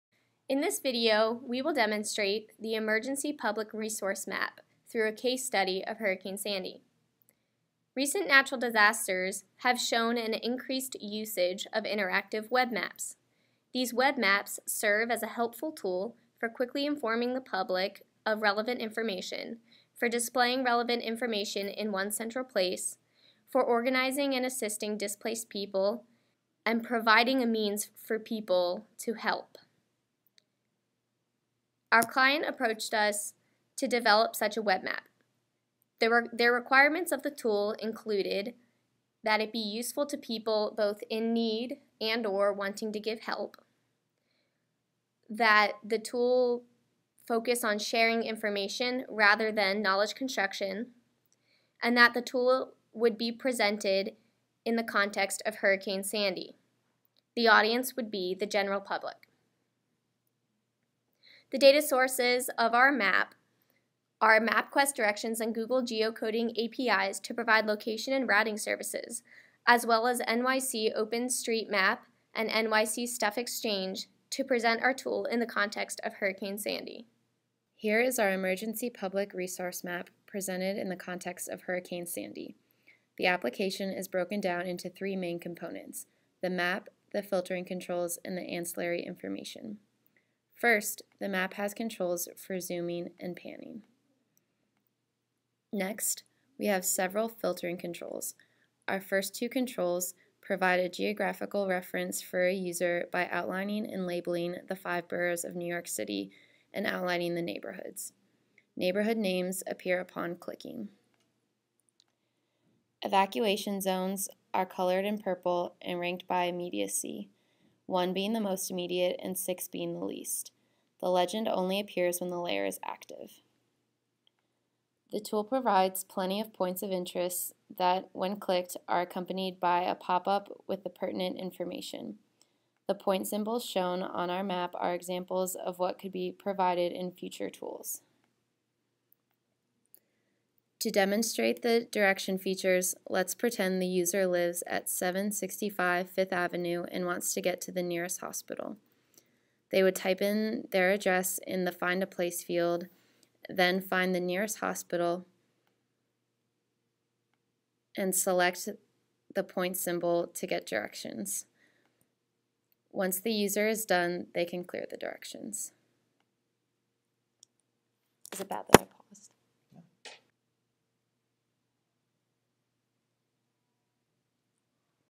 Hurricane Sandy Public Resource Map
Hurricane Sandy Public Resource Map is a web resource for those affected by, or providing assistance during a crisis to locate themselves and find their way to emergency resources, using Hurricane Sandy as a case study. It was developed by Anne Harris, Katherine Meckler, Gregory Milbourne as a class project for Dynamic Cartographic Representation (Geog 461W), offered at Penn State Geography and taught by Dr. Alan MacEachren. Geog 461W projects focus on design and production of a dynamic, map-based, web-capable geospatial tool targeted to support some clearly articulated objectives.